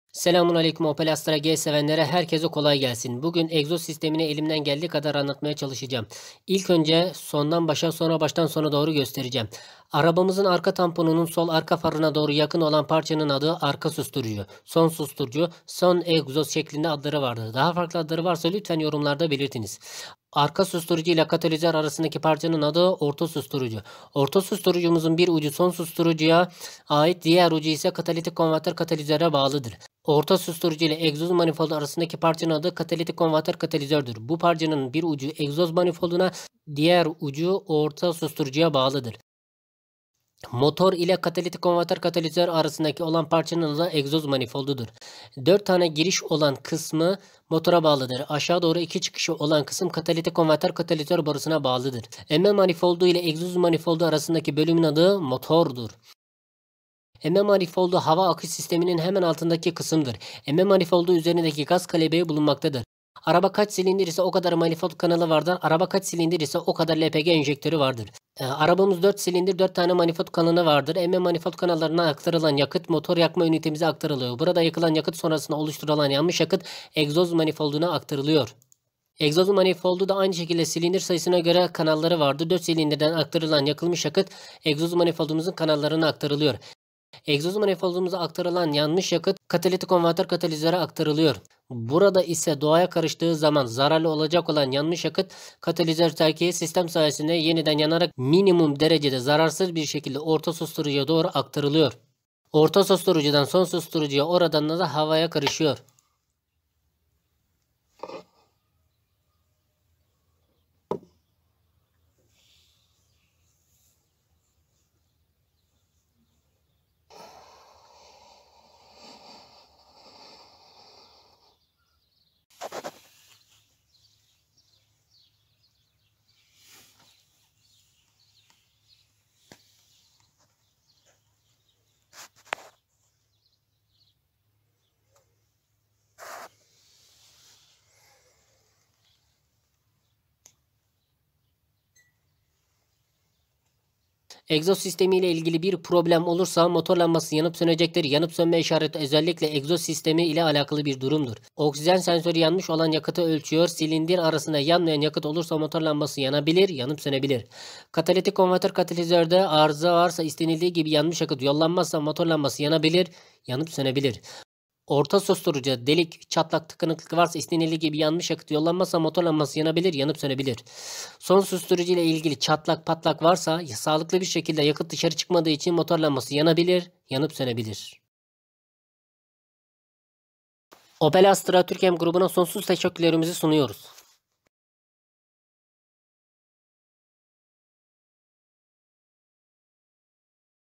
EGZOZ SİSTEMİ NEDİR OPEL ASTRA G
opel astra g arabamıza ait egzoz sistemini ele aldım ve sizlere anlattım inşallah yararlı olmuştur :)